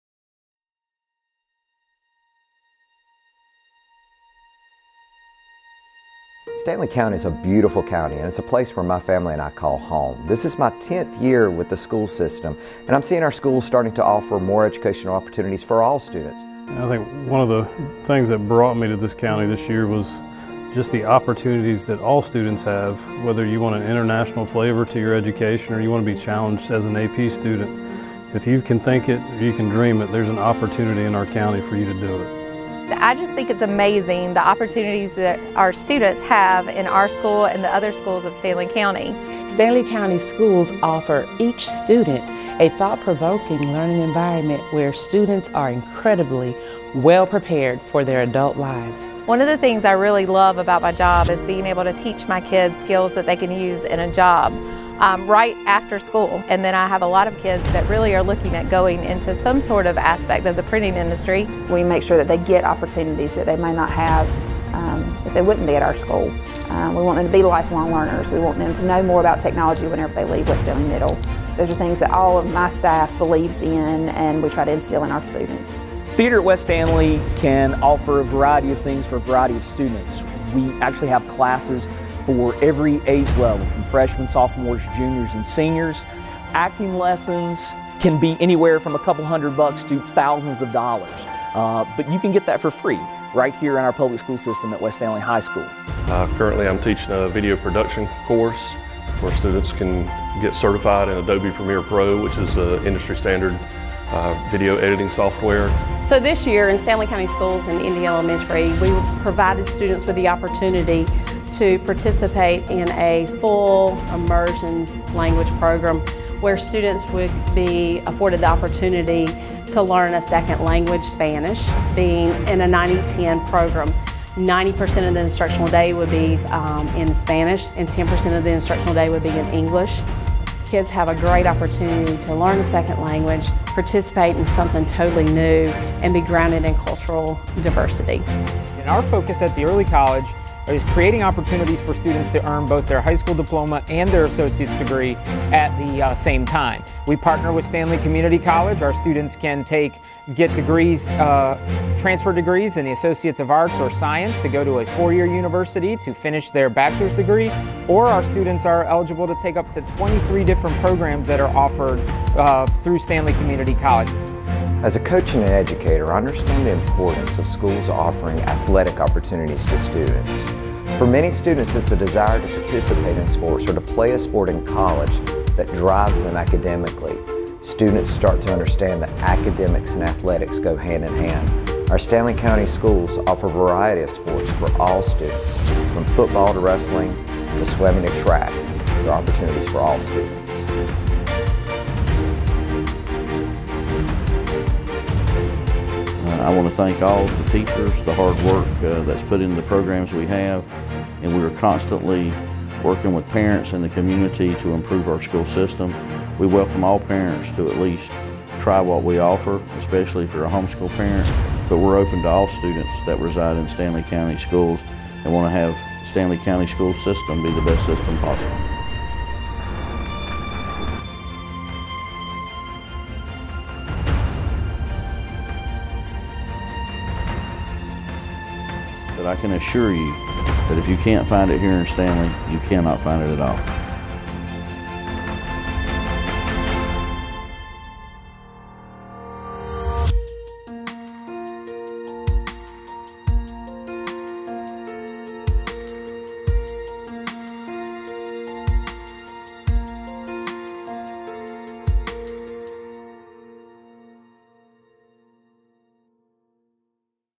Stanly County Schools Interviews Promo Video 2
Check out our video with principal interviews on what makes Stanly County Schools a great place.